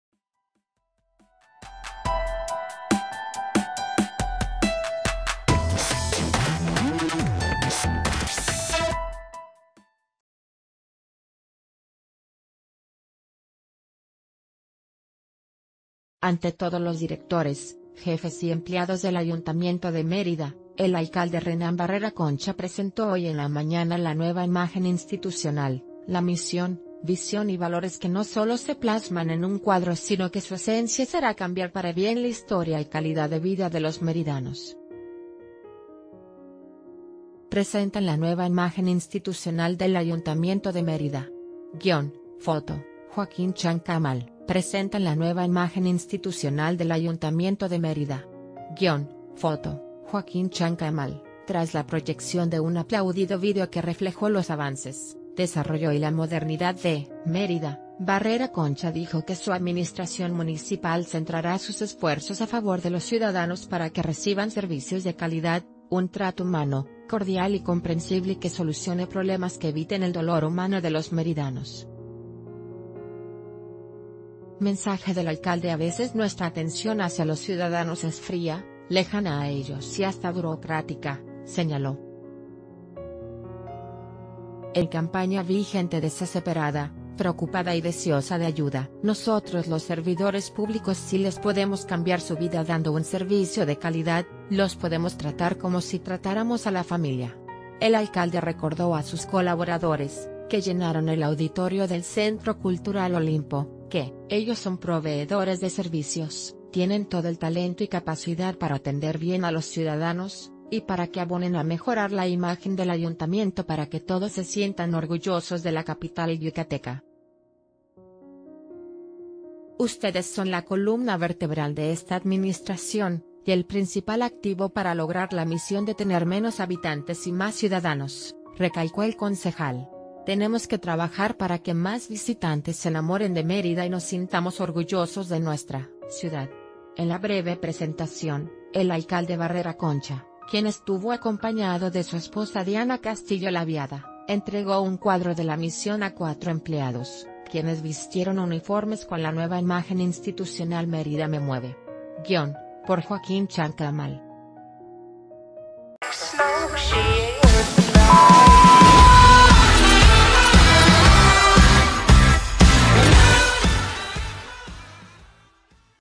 Presentan la nueva imagen institucional del Ayuntamiento de Mérida
Ante todos los directores, jefes y empleados del Ayuntamiento de Mérida, el alcalde Renán Barrera Concha presentó hoy en la mañana la nueva imagen institucional, la misión, visión y valores que no sólo se plasman en un cuadro sino que su esencia será cambiar para bien la historia y calidad de vida d...